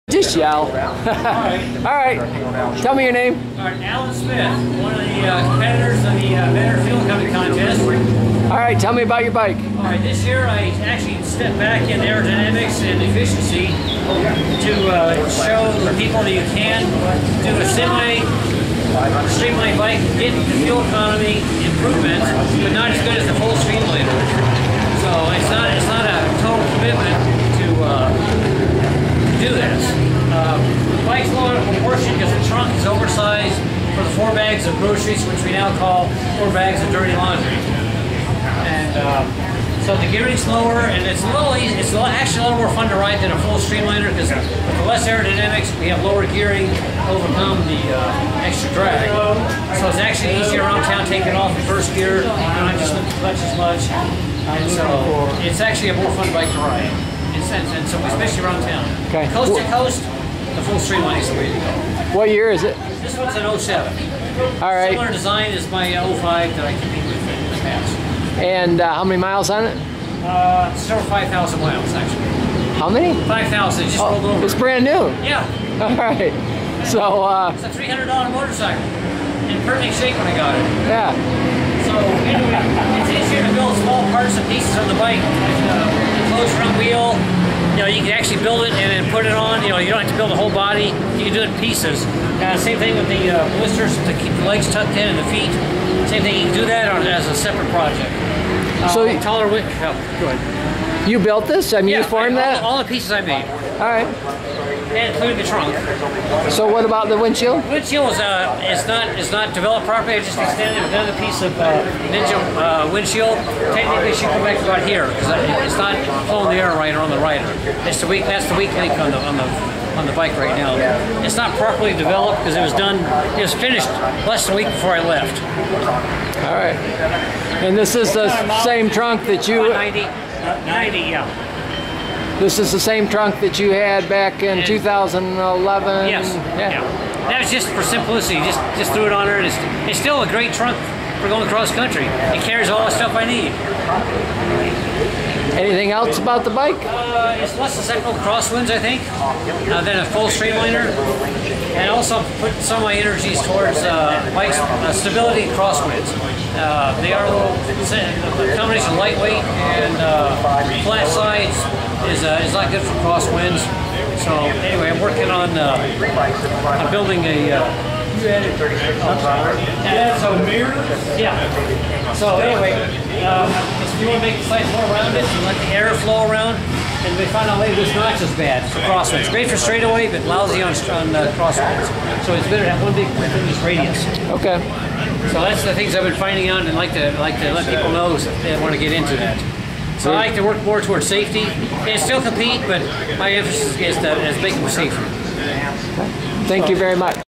Vetter Challenge 2017 - Alan Smith Interview
Alan Smith Talks about his bike for 2017